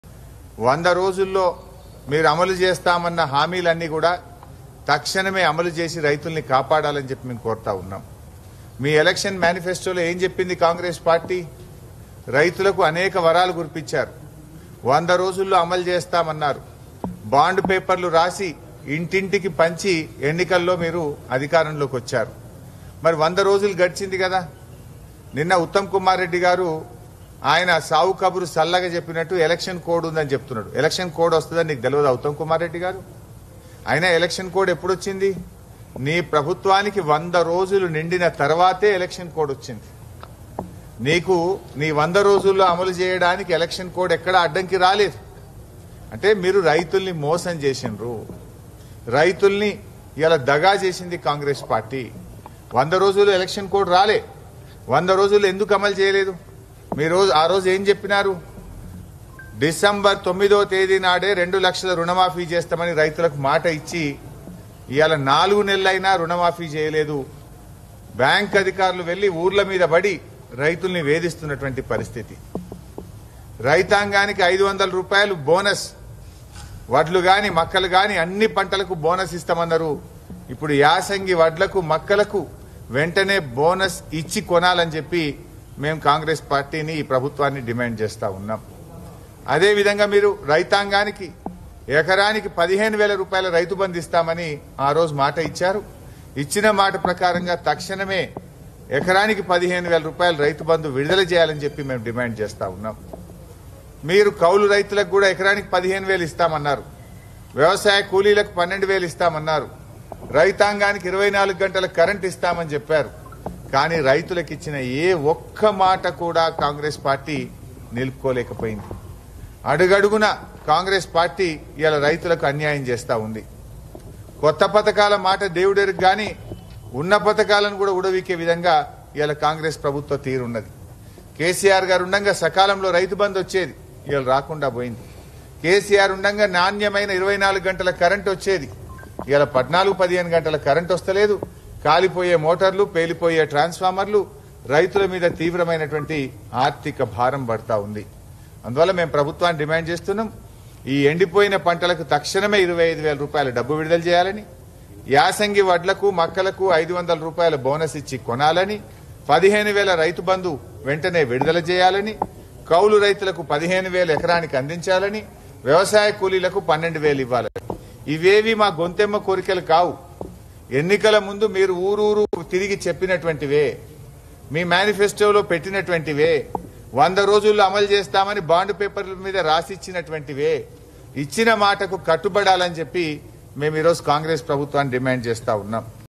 100 రోజుల్లో అమలు చేస్తానన్న హామీలను ఎందుకు అమలు చేయలేదు : Harish Rao | Ntv
For more latest updates on the news :
100 రోజుల్లో అమలు చేస్తానన్న హామీలను ఎందుకు అమలు చేయలేదు : Harish Rao | Ntv